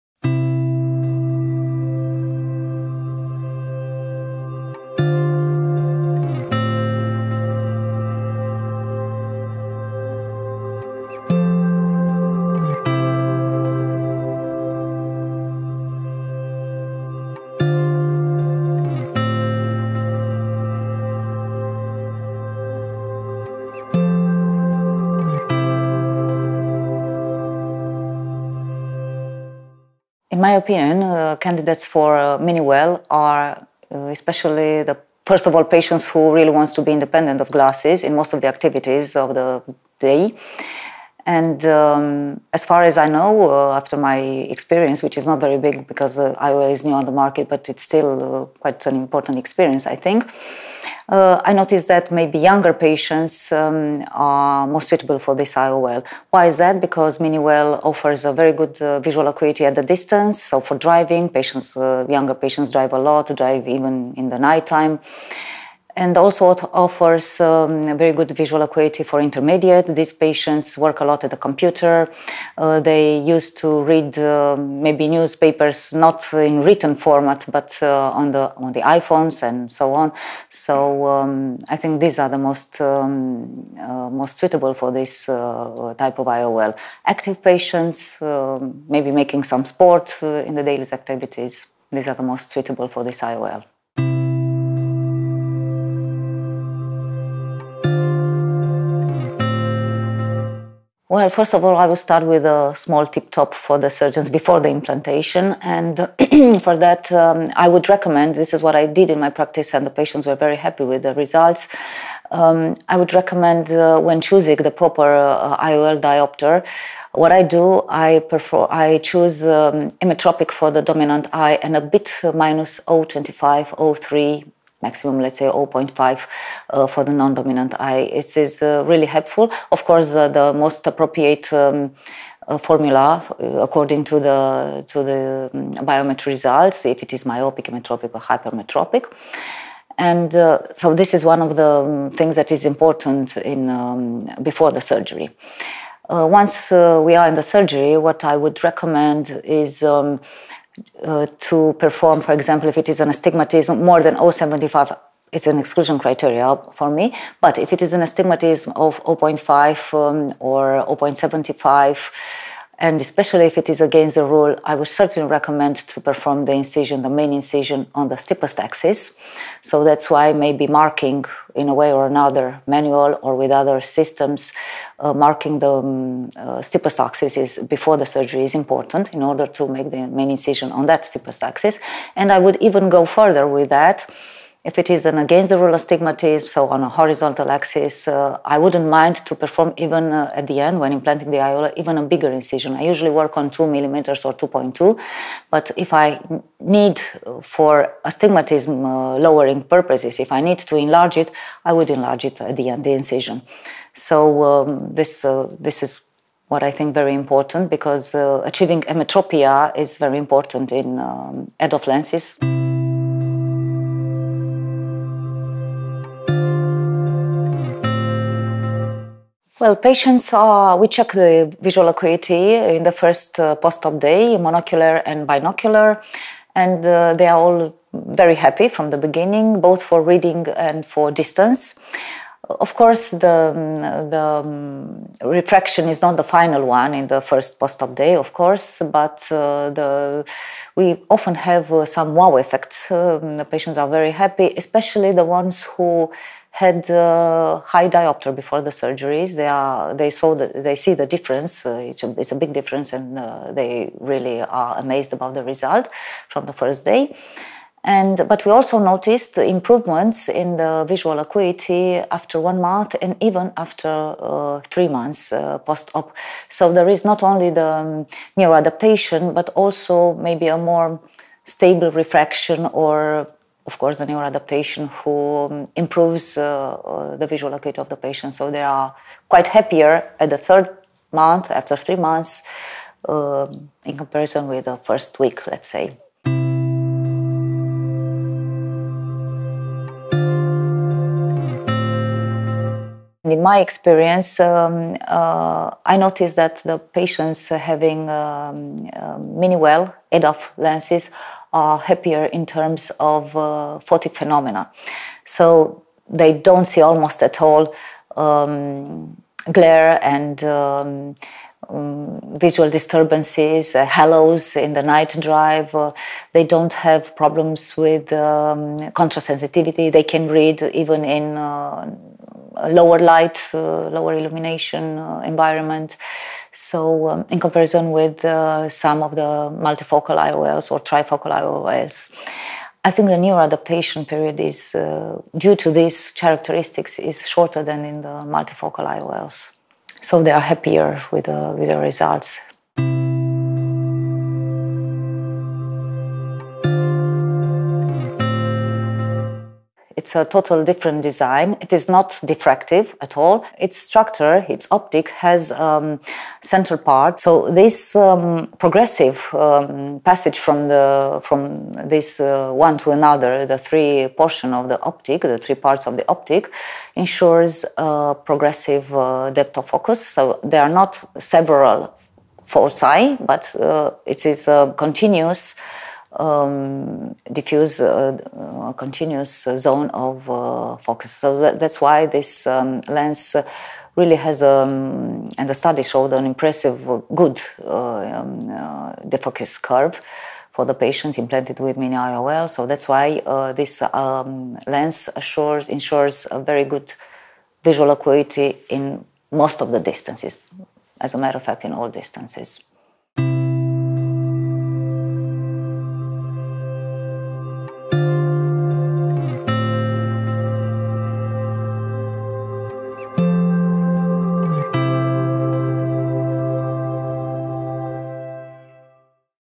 In Conversation With… Ozana Moraru
We sat down in Lisbon with Ozana Moraru and had a conversation about SiFi’s MINI WELL extended depth of focus intraocular lens – everything from how it works, patient selection, best practice for implantation, how the lens compared with other EDOF IOLS and what patients outcomes (and reactions) are like!